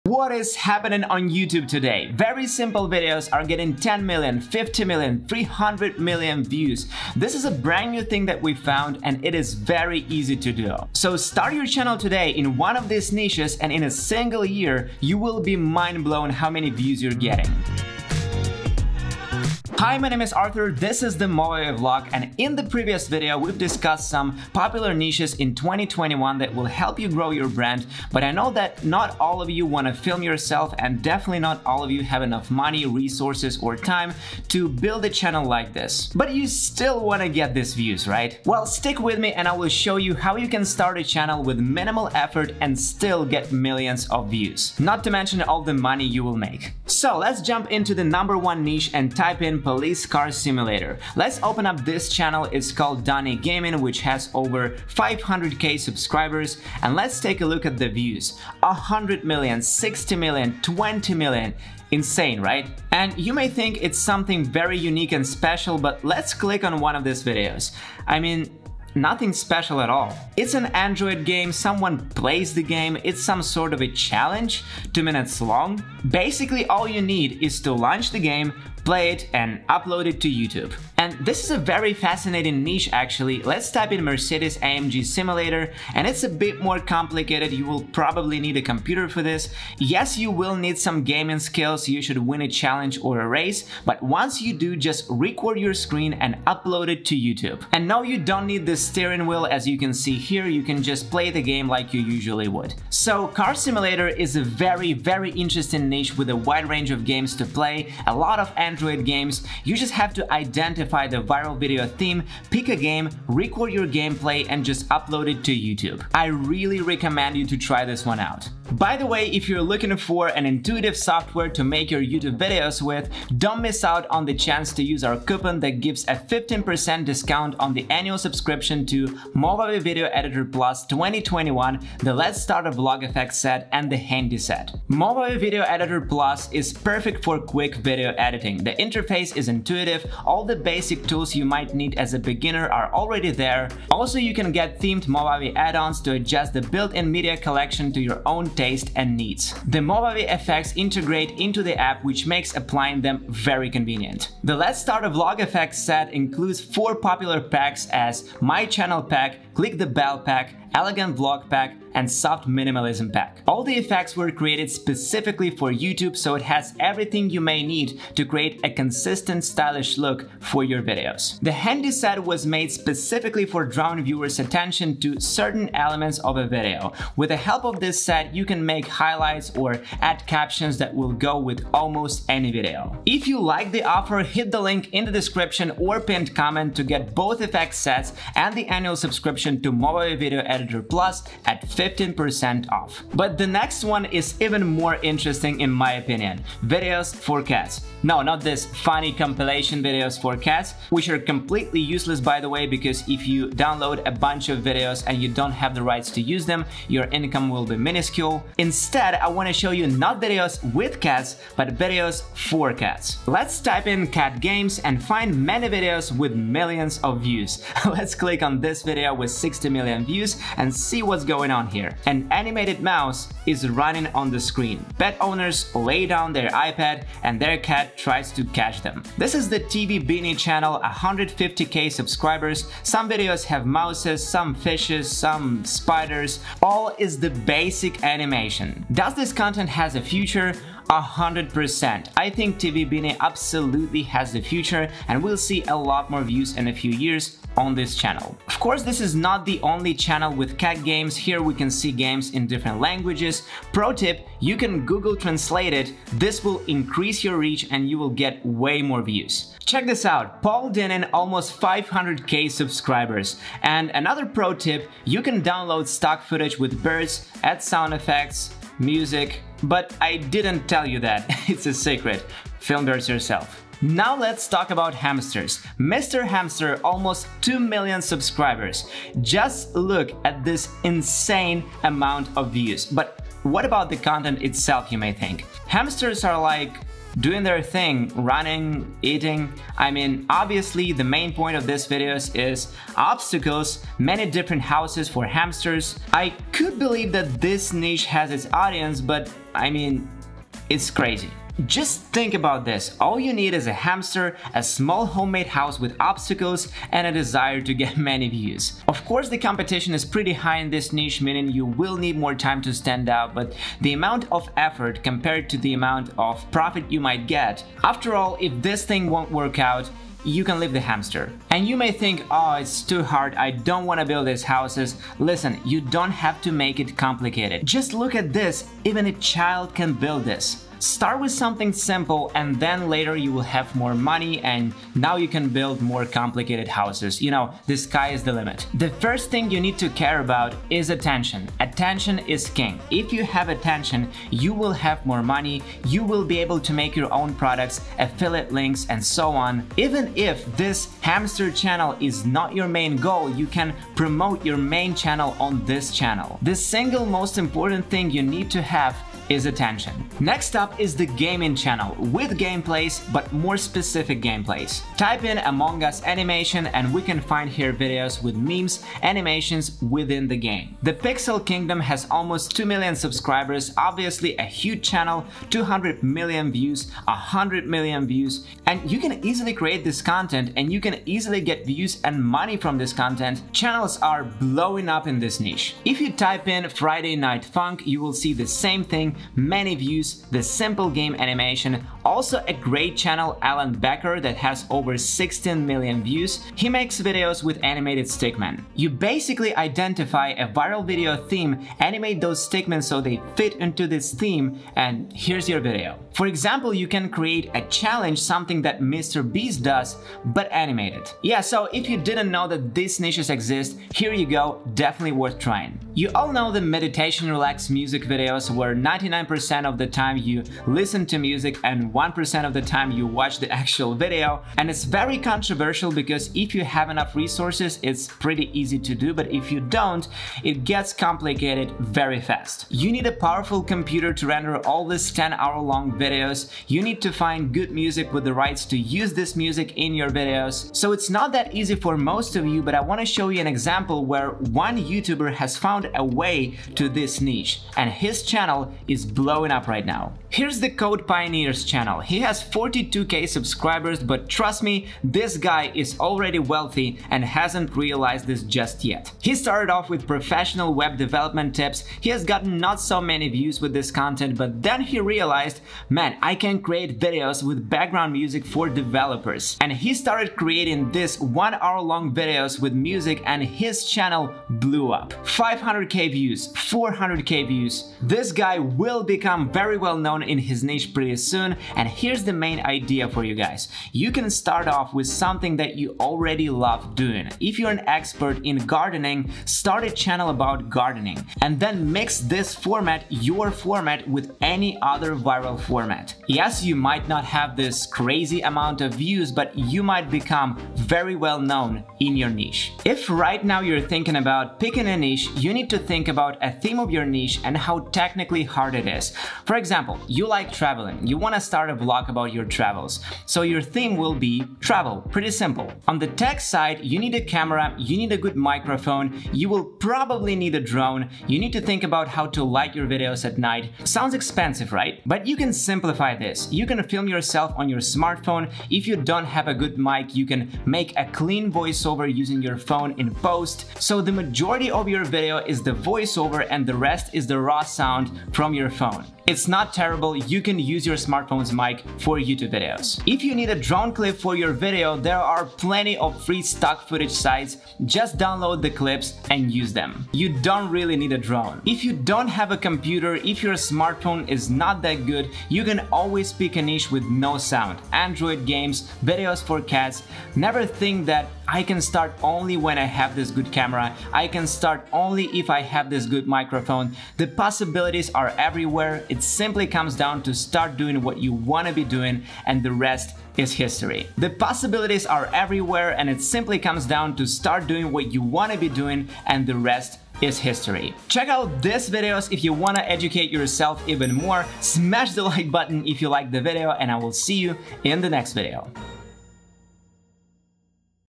How To Make $3398 By 1-Minute Youtube Video With No Subscribers?
How to make money on Youtube by making 1-minute videos on your channel? No matter how much subscribers you have, because such videos going viral easily and fast.